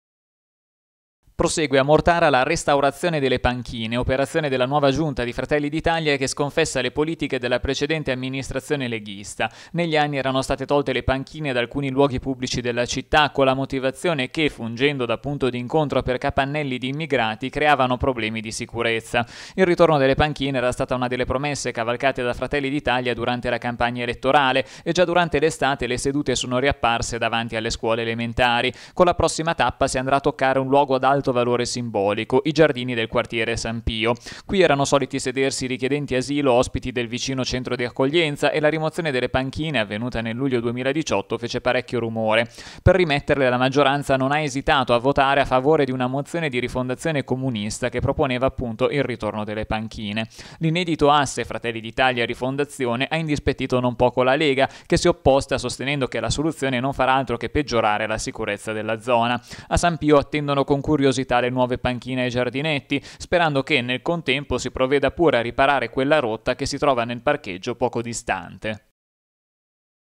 Mortara, Fratelli d'Italia rimette le panchine che la Lega aveva tolto
Prosegue a Mortara la restaurazione delle panchine, operazione della nuova giunta di Fratelli d’Italia che sconfessa le politiche della precedente amministrazione leghista. Negli anni erano state tolte le panchine da alcuni luoghi pubblici della città, con la motivazione che, fungendo da punto di incontro per capannelli di immigrati, creavano problemi di sicurezza. Il ritorno delle panchine era stata una delle promesse cavalcate da Fratelli d’Italia durante la campagna elettorale, e già durante l’estate le sedute sono riapparse davanti alle scuole elementari. Con la prossima tappa si andrà a toccare un luogo ad alto valore simbolico: i giardini del quartiere San Pio. Qui erano soliti sedersi i richiedenti asilo ospiti nel vicino centro d’accoglienza e la rimozione delle panchine, avvenuta nel luglio 2018, fece parecchio rumore. Per rimetterle, la maggioranza non ha esitato a votare a favore di una mozione di Rifondazione Comunista, che proponeva appunto il ritorno delle panchine. L’inedito asse Fratelli d’Italia – Rifondazione ha indispettito non poco la Lega, che si è opposta sostenendo che la soluzione non farà altro che peggiorare la sicurezza della zona. A San Pio attendono con curiosità le nuove panchine ai giardinetti, sperando che, nel contempo, si provveda pure a riparare quella rotta che si trova nel parcheggio poco distante...